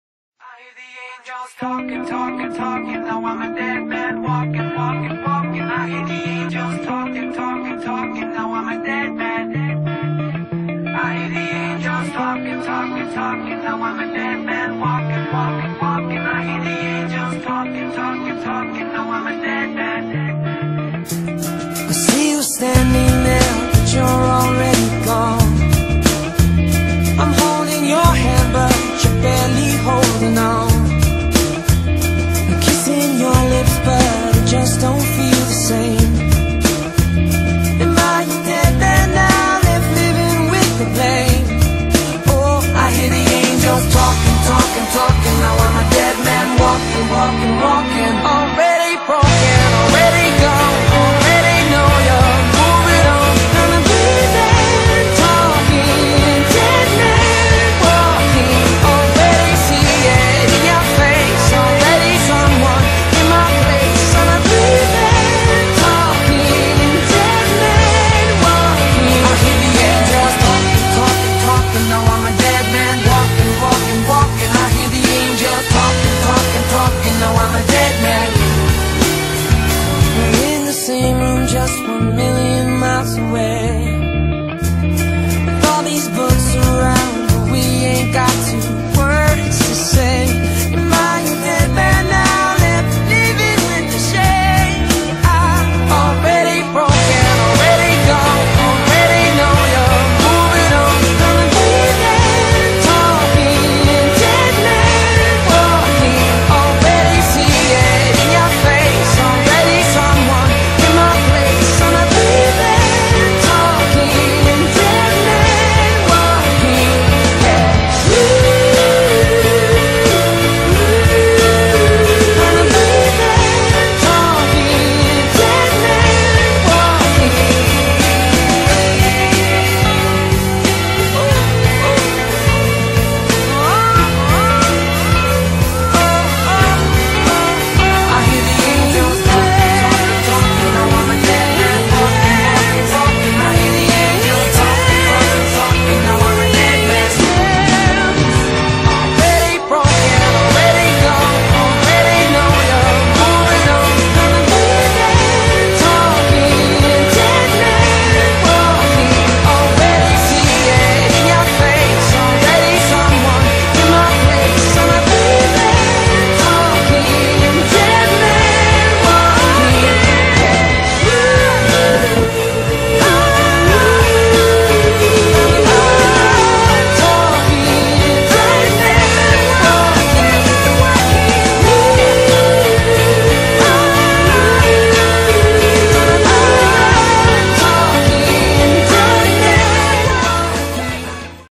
"I hear the angels talking"
First time working Flynn hard since the dreadful snow; hacked out for about an hour and schooled him in a field on the hack for about 20 - 30 mins - we will both feel it tomorrow! Haha i love my beautiful horse, so well behaved in every manor and tries hard in everything he does, and it is really starting to pay off! Going to start eventing him this year and will be XC training him a Lincomb soon too, i will upload a video when i do so :)